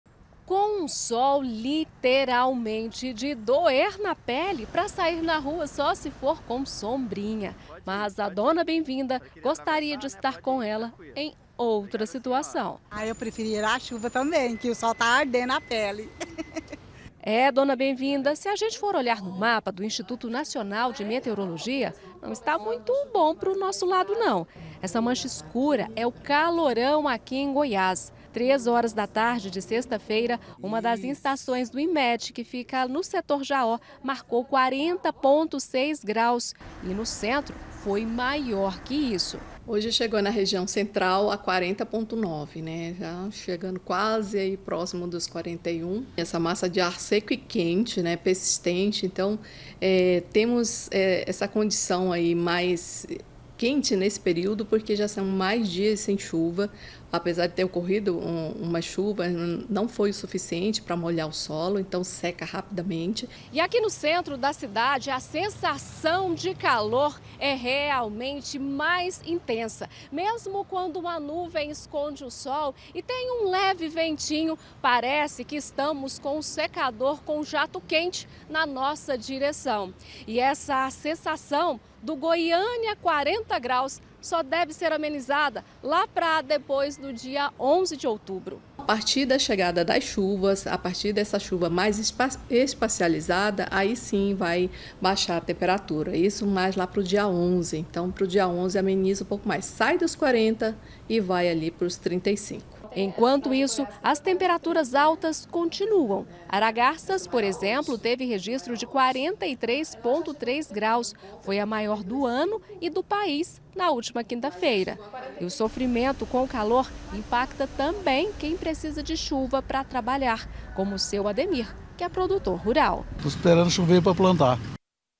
GOIÂNIA REGISTRA NOVO RECORDE DE CALOR
O CALOR BATEU HOJE 41 GRAUS EM GOIÂNIA. UM NOVO RECORDE. EM ARAGARÇAS, FOI PIOR AINDA, 42 GRAUS.